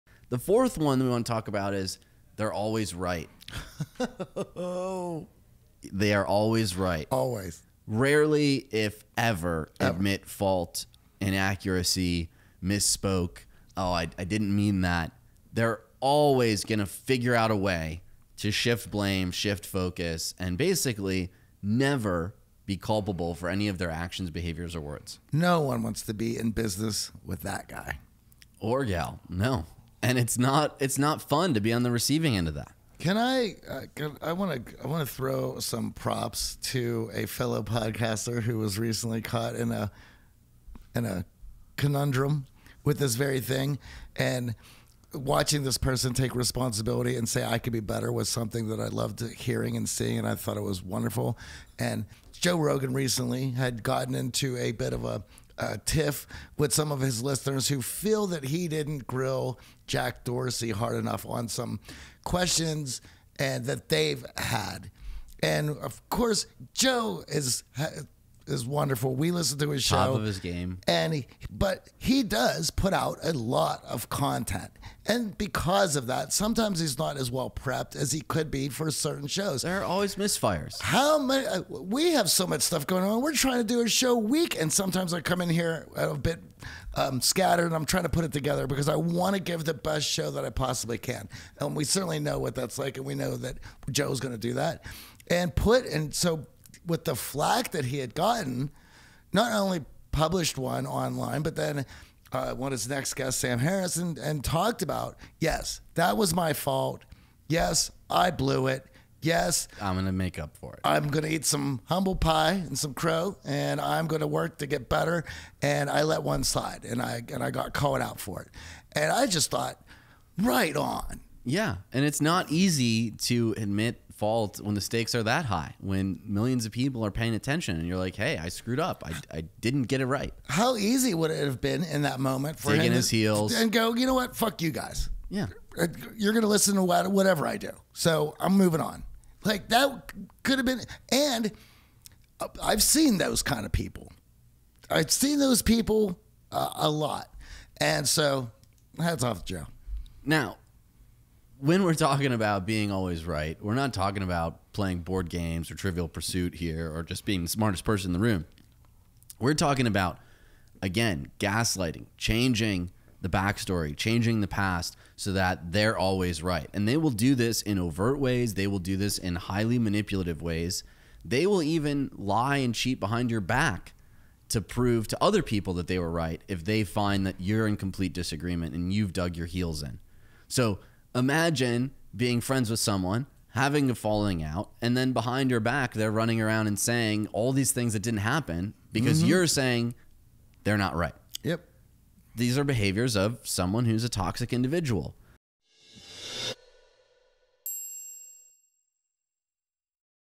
A Quick Tip on Spotting Toxic People
It's not always easy to identify toxic people. In this excerpt from Podcast 748, AJ and Johnny discuss one of the seven main qualities that toxic people often have, as well as a way to avoid falling into this mindset.